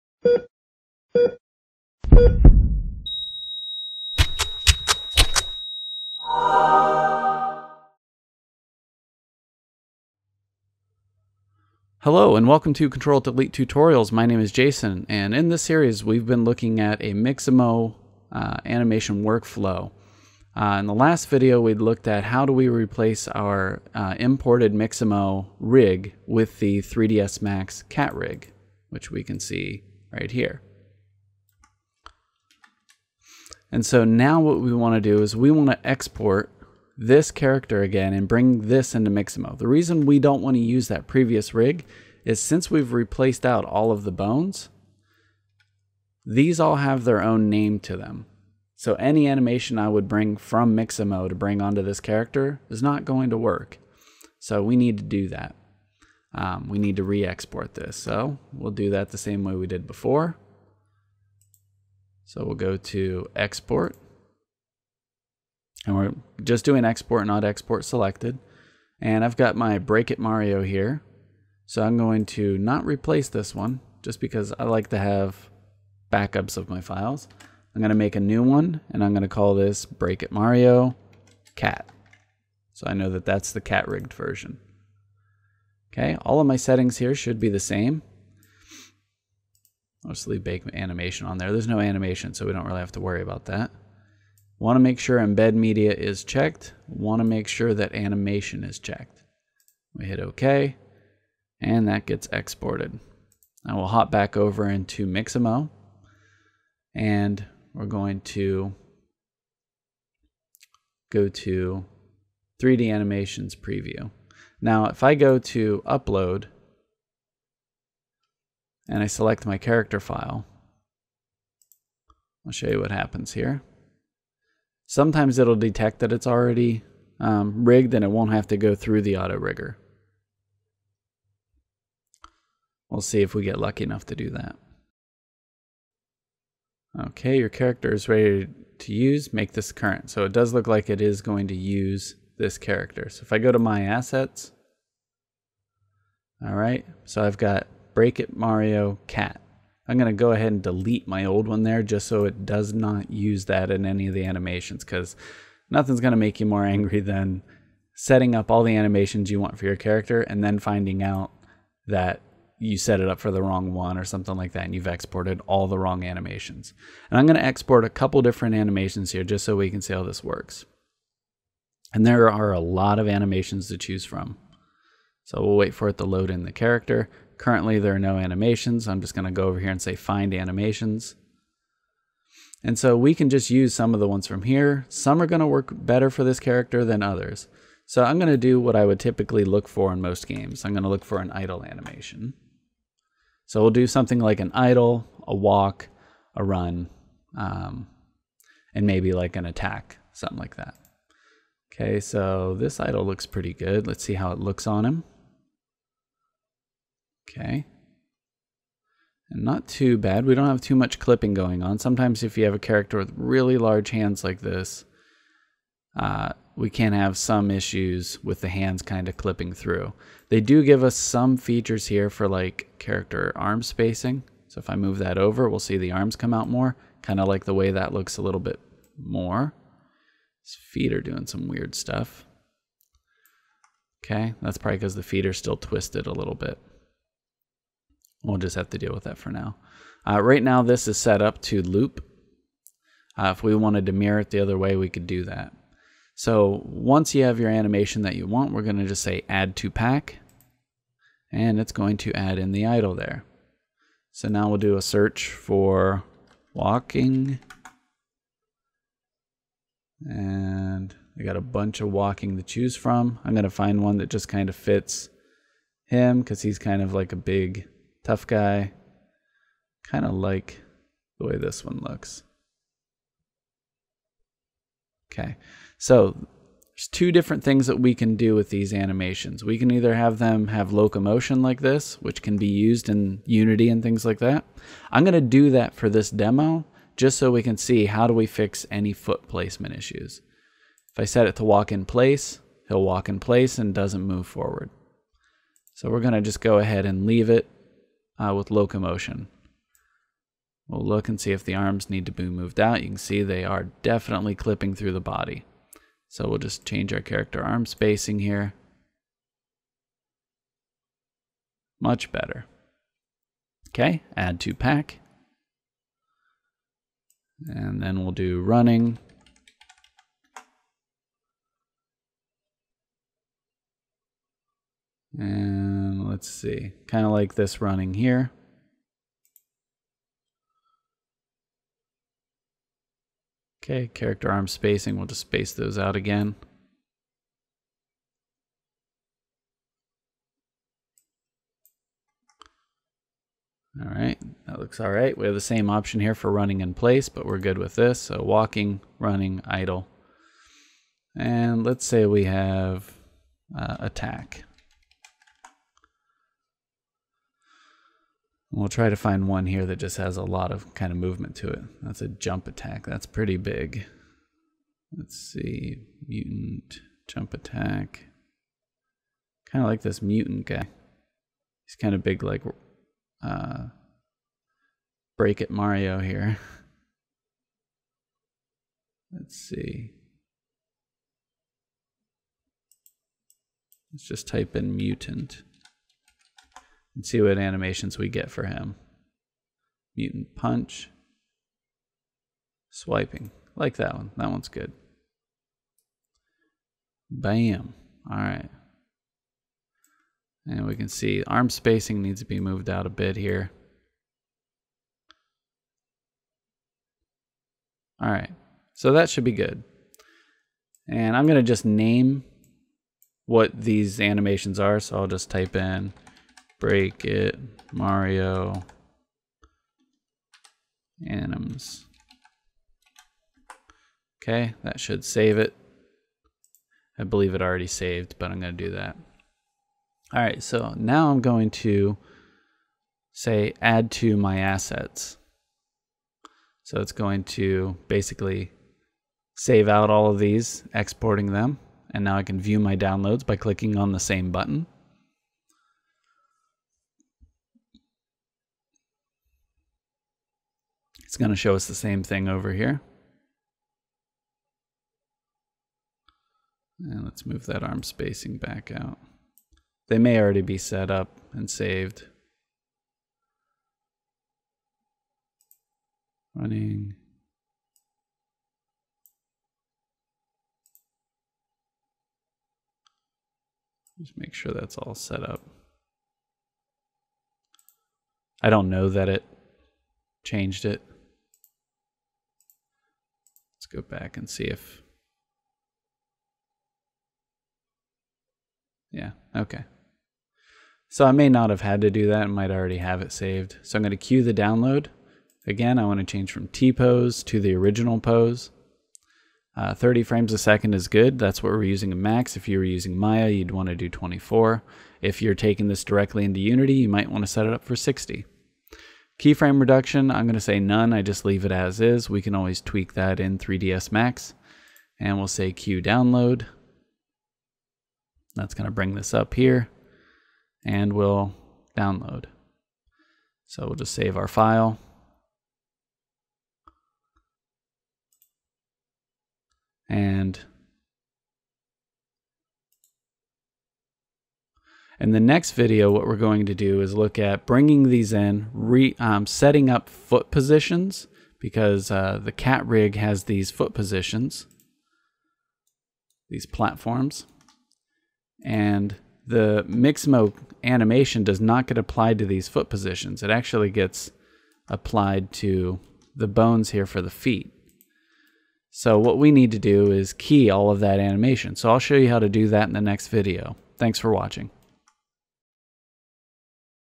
The Mixamo Workflow - Using Mocap with the 3ds Max CAT Rig - Part 3 of 6
This series of tutorials covers a workflow I created for working with Mixamo motion capture data and applying their auto rigged animations to the 3ds Max CAT rigging system. These videos will show you how to take their animations and how to edit them to fit any size or any shaped character.

Here is a link to my Google Drive location to get the files: